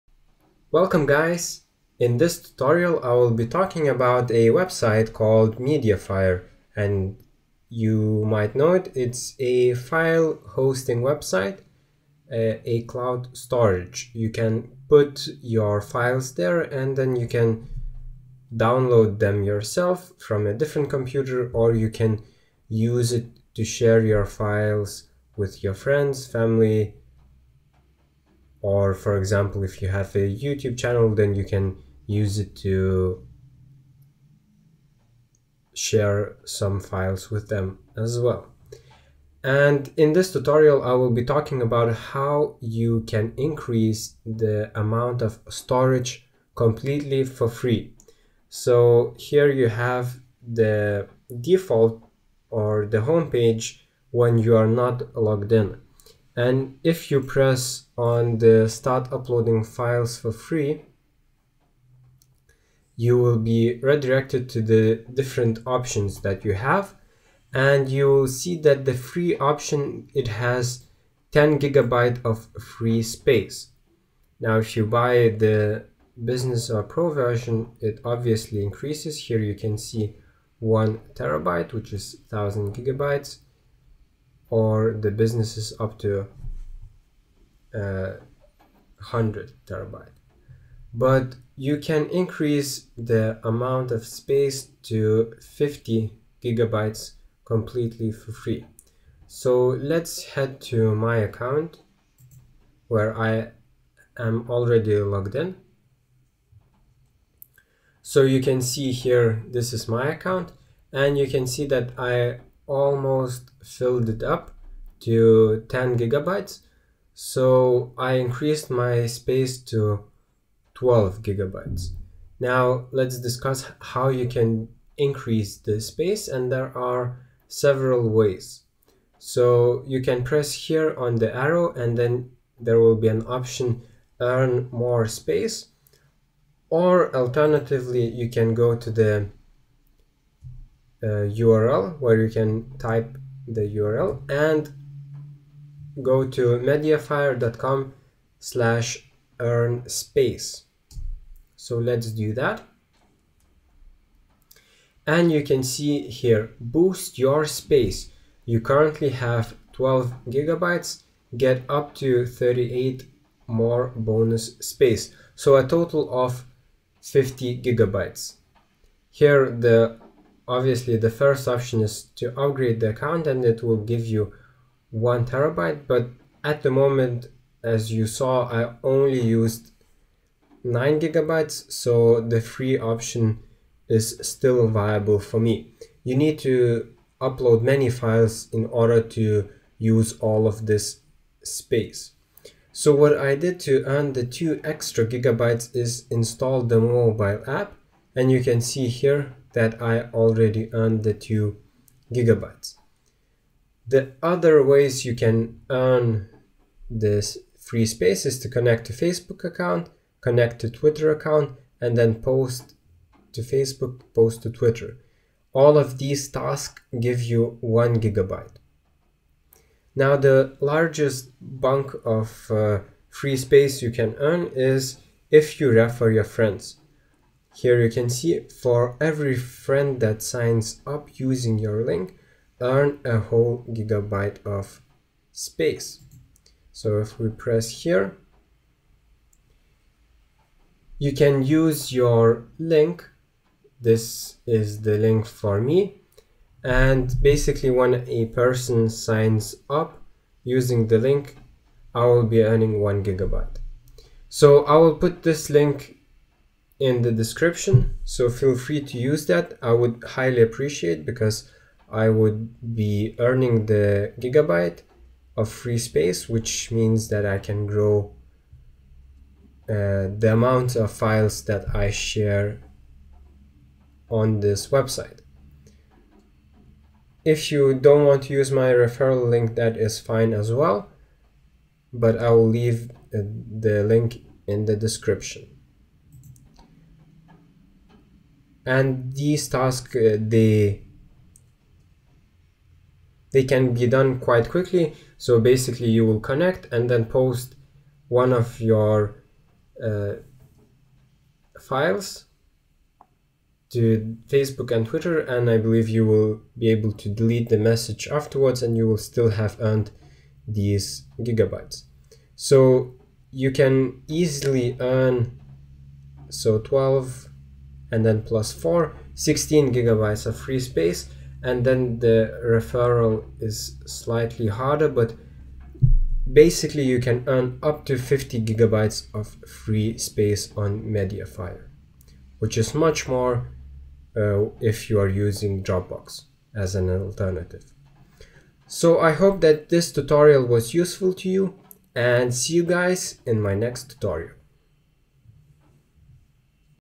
How to Increase Mediafire Space to 50 GB for Free
In this tutorial I discuss various ways how to increase your Mediafire storage for free.